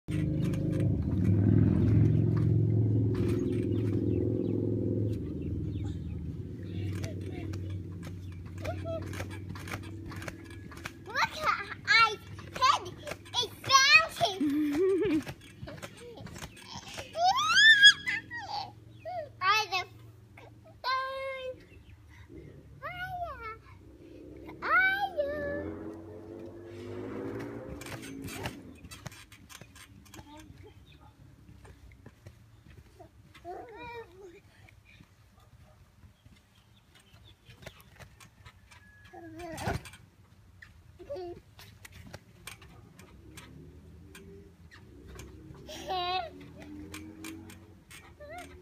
Trampoline
Braithe and Isla trying the trampoline out